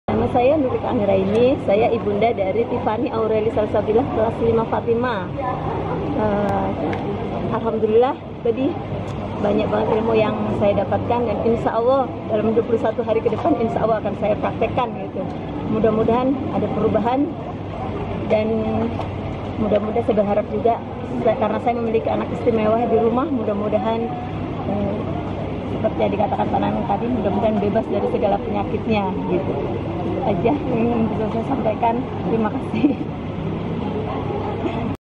TESTIMONI PESERTA PARENTING THE7AWARENESS
Seminar Parenting The7awareness di SDIT DAUROH

Setelah sesi pertama untuk walimurid kelas 1.2,3 sukses awal Oktober lalu hari ini giliran angkatan ke2 untuk orangtua siswa kelas 4,5,6 SDIT DAUROH Tangerang, sebanyak 300 peserta sangat antusias belajar bersama sama tentang 7 terapi kesadaran bagaimana menjadi orang tua diatas rata-rata